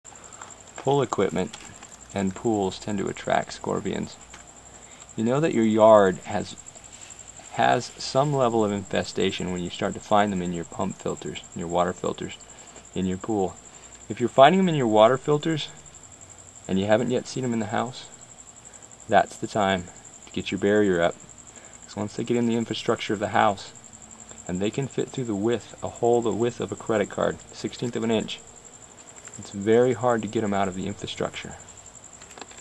Phoenix Scorpion Control - Scorpion Infested Pool Equipment | Bulwark Exterminating
Phoenix Scorpion Control Scorpion Infested Pool Equipment is the first sign you need treatment. Info on Scorpions from the Scorpion Gurus themselves.